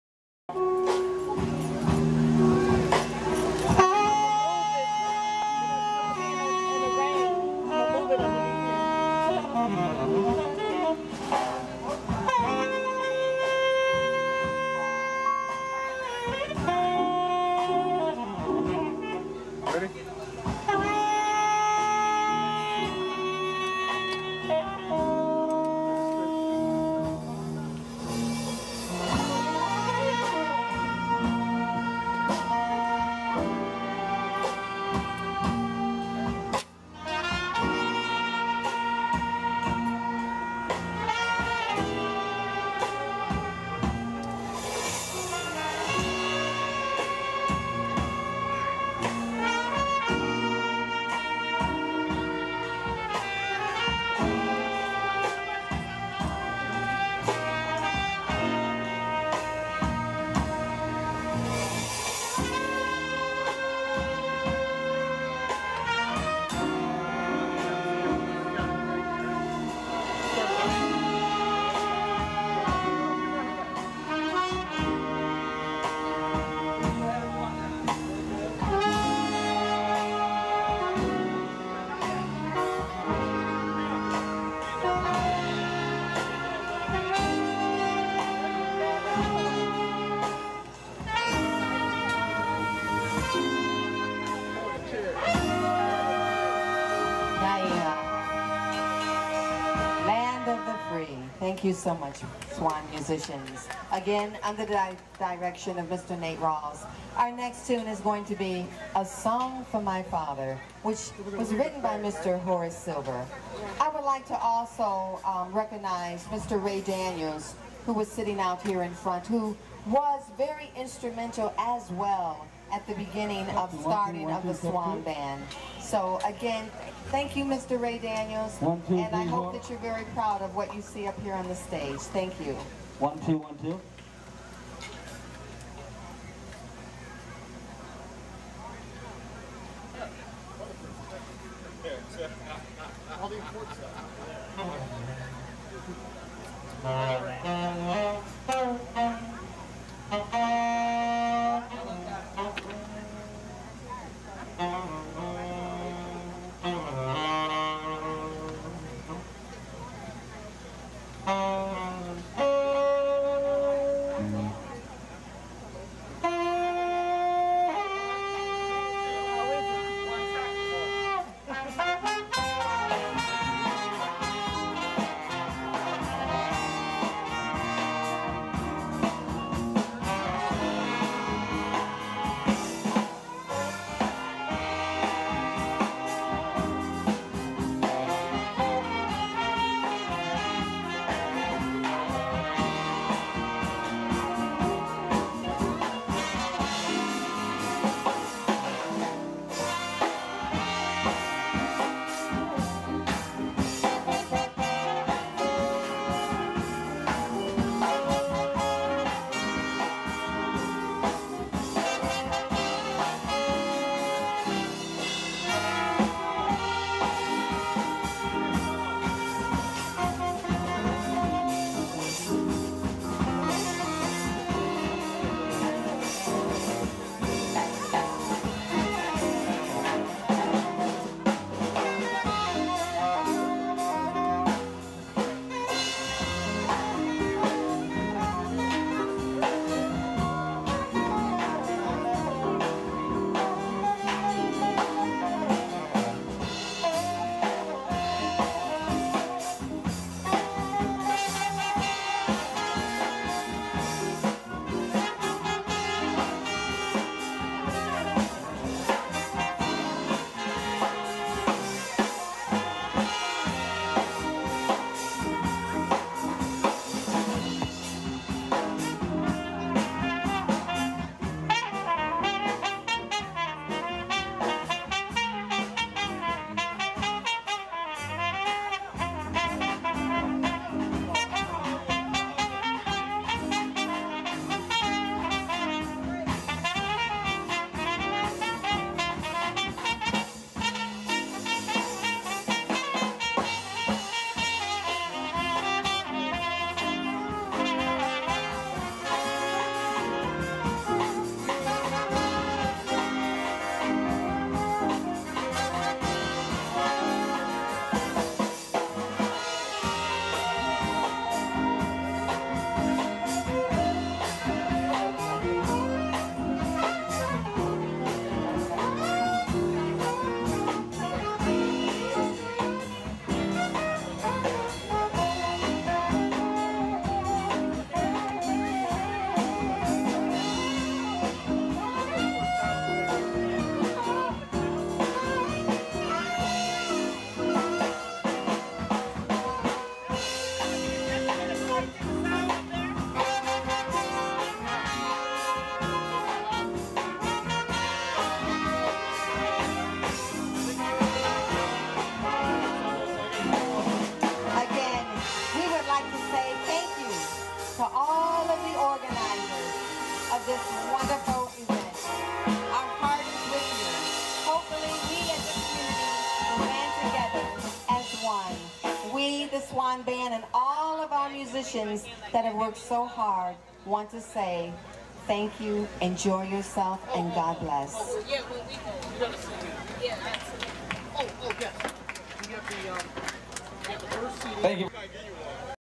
Rochester 2010 Jueteenth Celebration 6-Nate Rawls/SWAN Band pt 2
Conclusion of set by Nate Rawls and the SWAN Community Band at Juneteenth in 2010.